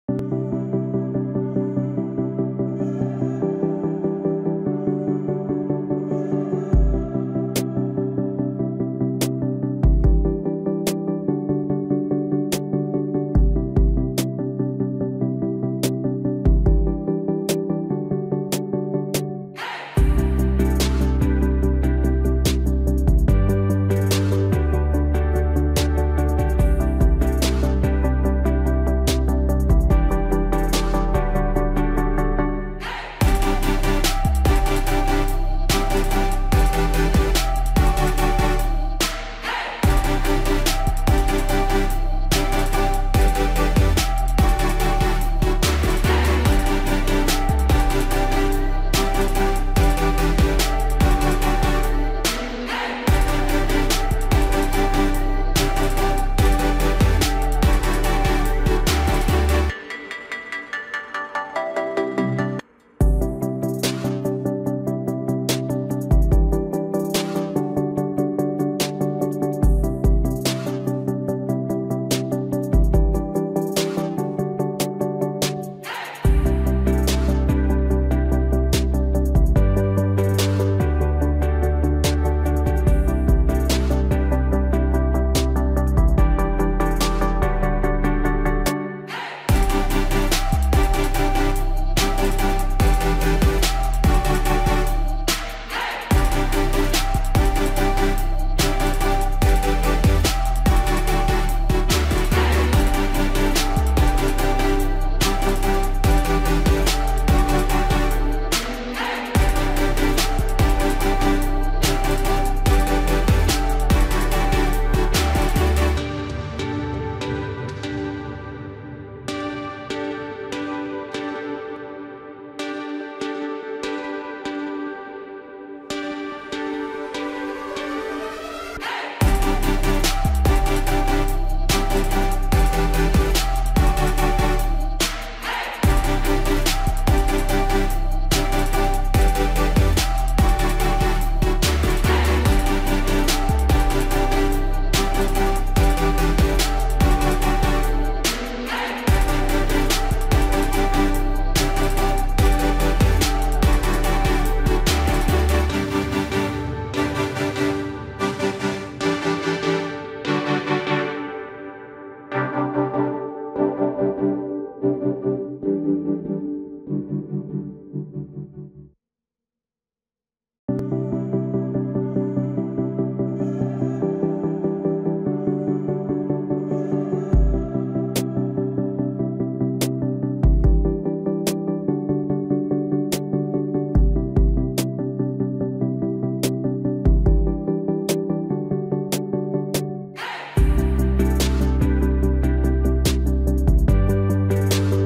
LEGO instructions - Super Heroes - 76041 - The Hydra Fortress Smash (Book 2)
How to build the 2015 Lego Set 76041 - The Hydra Fortress Smash